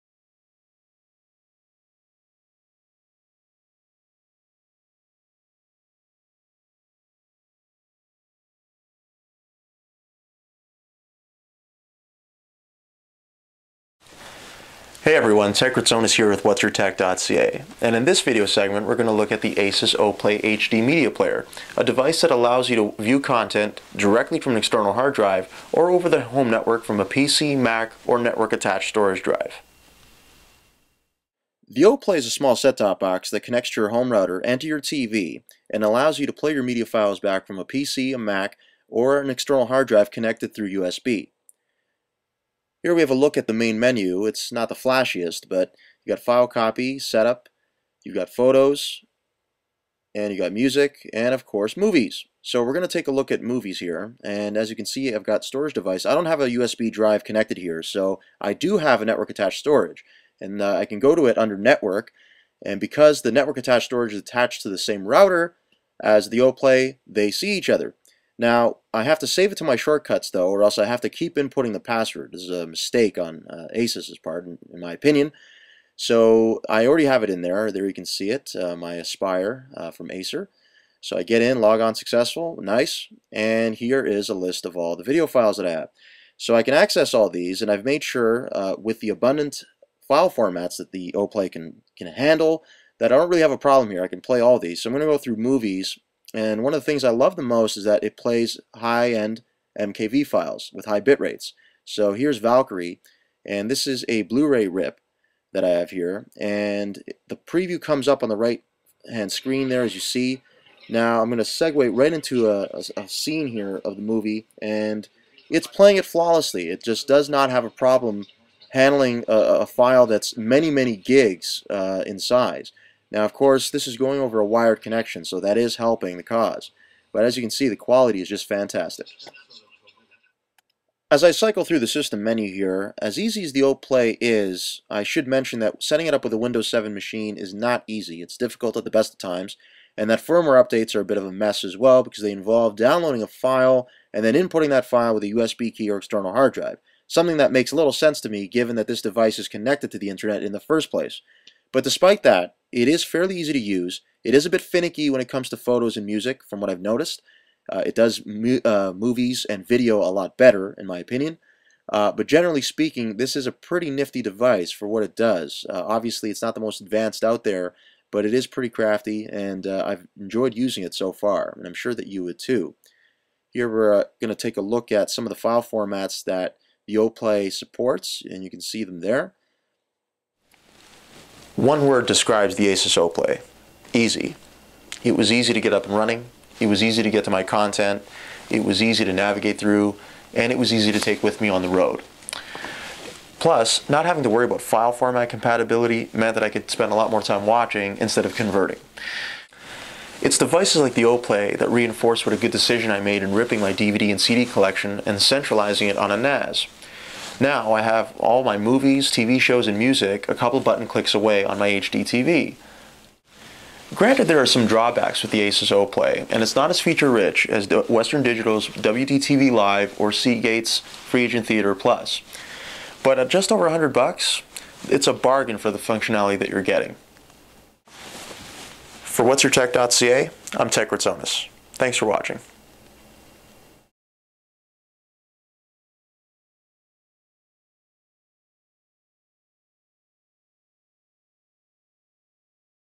Reviewing the ASUS O!Play HDP-R1 Media Player
WhatsYourTech.ca guru, Ted Kritsonis, reviews the ASUS O!Play HDP-R1, a small set top box that connects to your home router and TV and allows you to enjoy your media files. You can also make use of it without an Internet connection by just plugging in an external hard drive via a USB or eSATA connection.